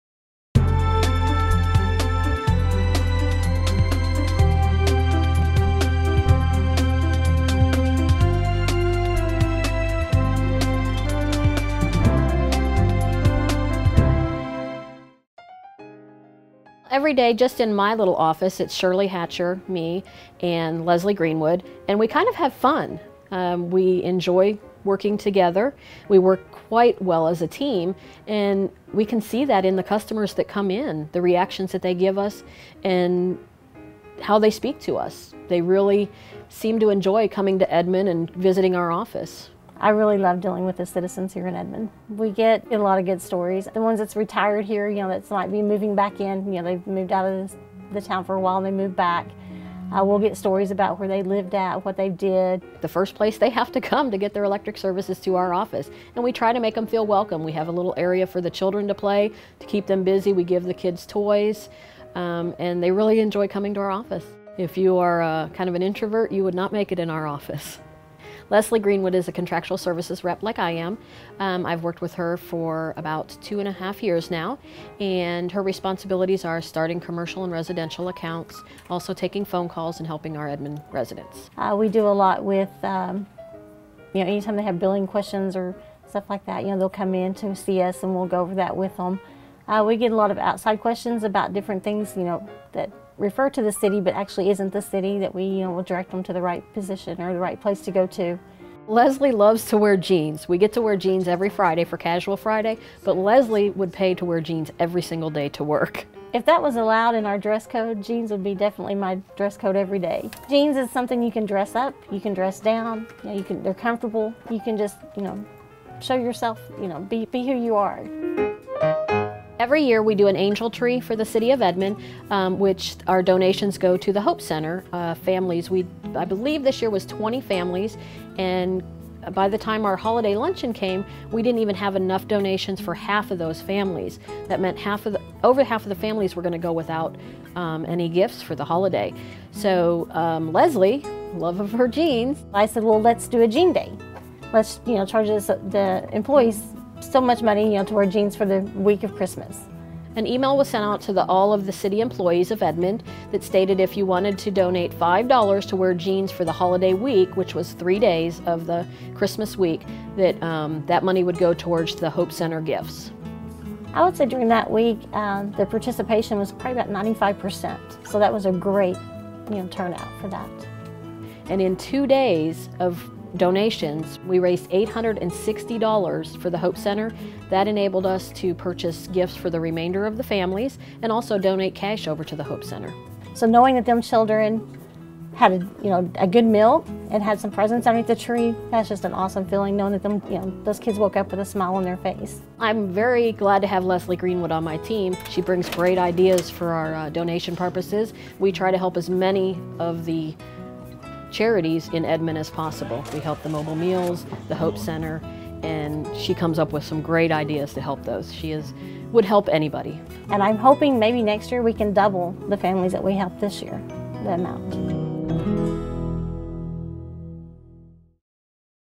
Employee Profile "Leslie Greenwood"
The City of Edmond has a lot of great employees.  This video tells you about one in the Utility Customer Service Department whose love for jeans helped many families in Edmond enjoy the holiday season.